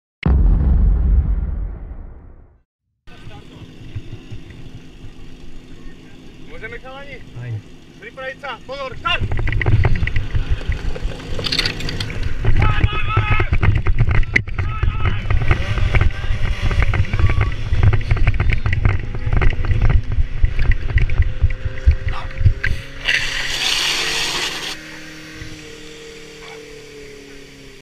DHZ Vrbové muži - PP
2.pokus, LP: 14,93s PP: 14,61s | 8.kolo IKHL v Cetune
Požičaný PP z DHZ Topolecká.